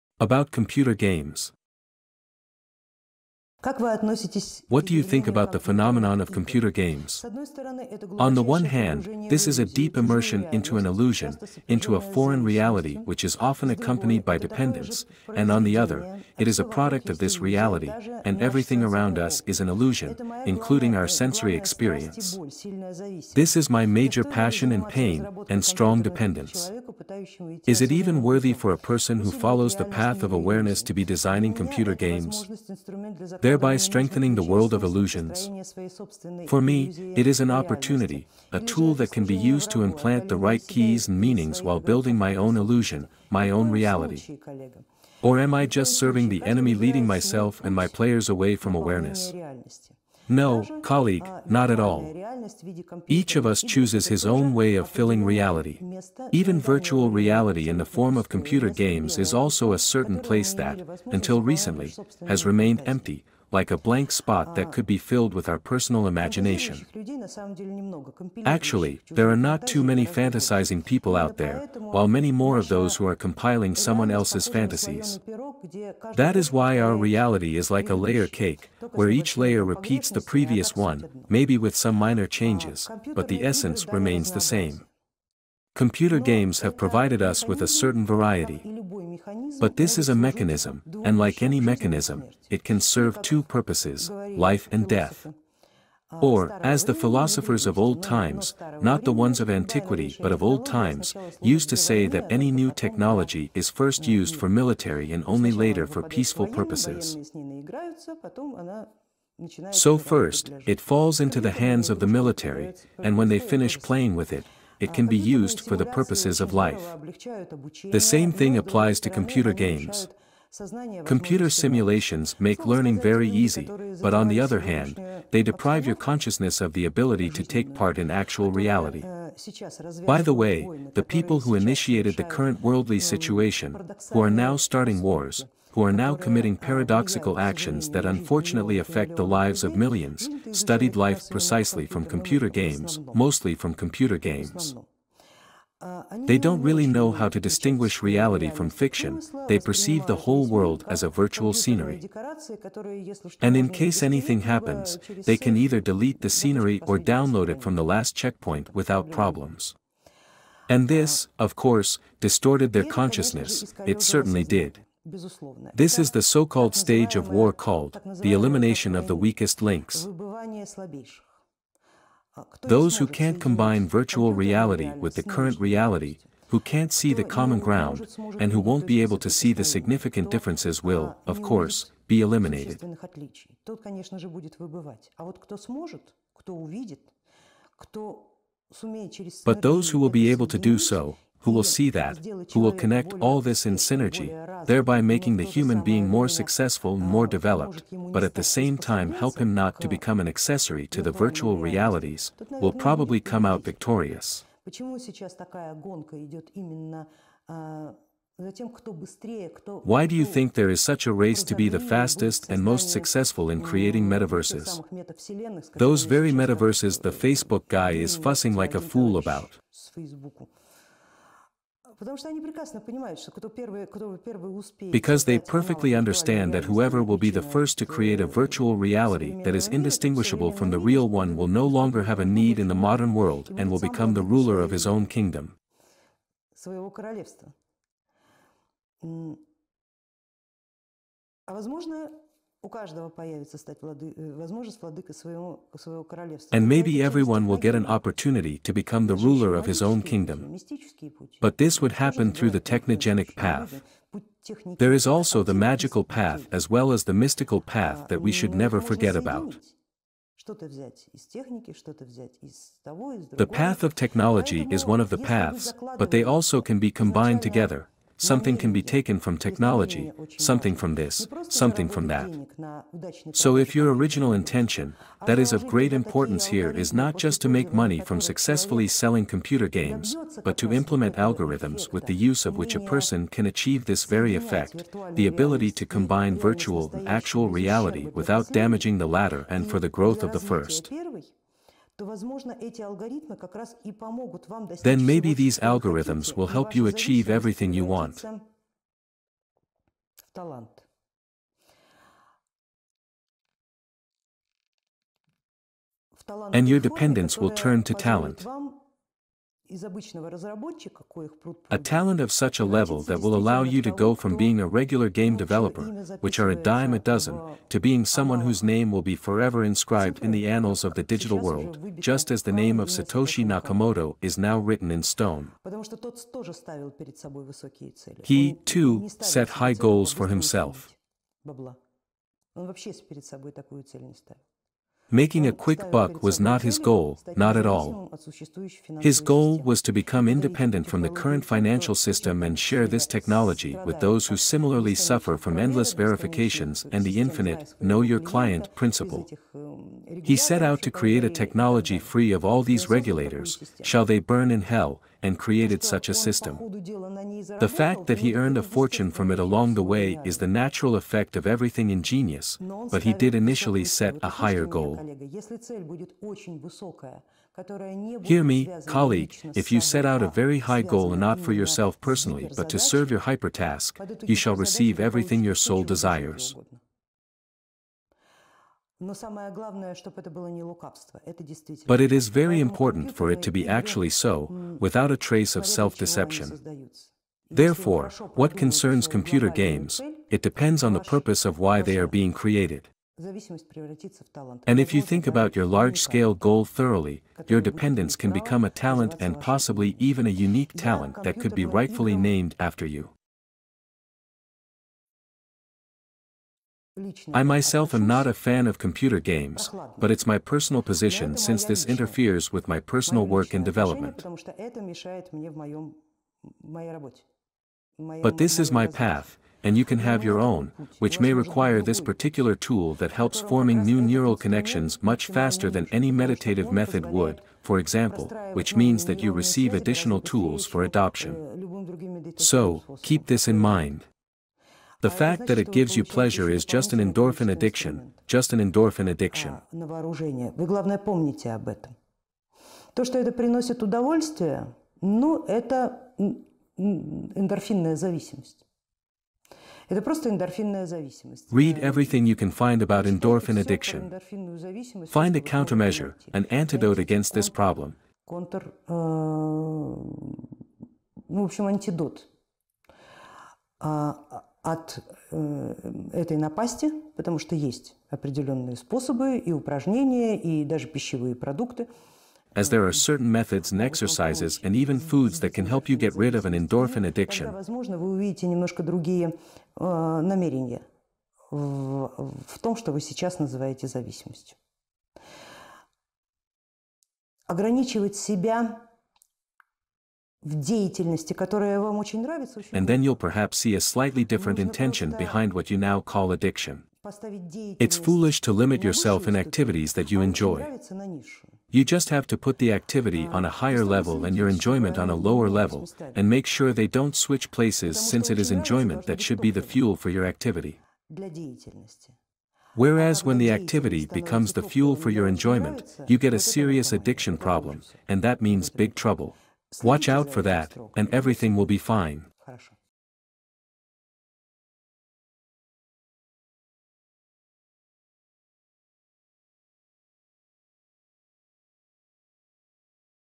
About Computer Games
About Computer Games. Each of us chooses his own way of filling reality. Even virtual reality in the form of computer games is also a certain place that, until recently, has remained empty, like a blank spot that could be filled with our personal imagination.
That is why our reality is like a layer cake, where each layer repeats the previous one, maybe with some minor changes, but the essence remains the same.
The philosophers of old times used to say that any new technology is first used for military and only later for peaceful purposes.
Computer games have provided us with a certain variety.
The same thing applies to computer games.
Computer simulations make learning very easy, but on the other hand, they deprive your consciousness of the ability to take part in actual reality.